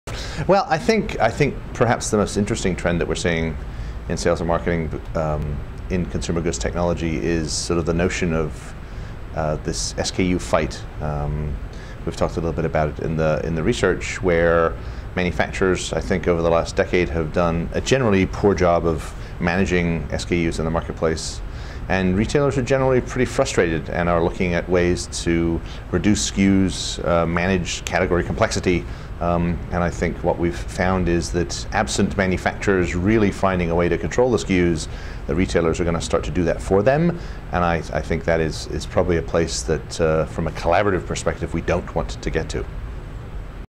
Simon Ellis, Practice Director, Supply Chain, IDC Manufacturing Insights
Industry expert Simon Ellis gives tactics for winning the escalating "SKU fight" between manufacturers and retailers.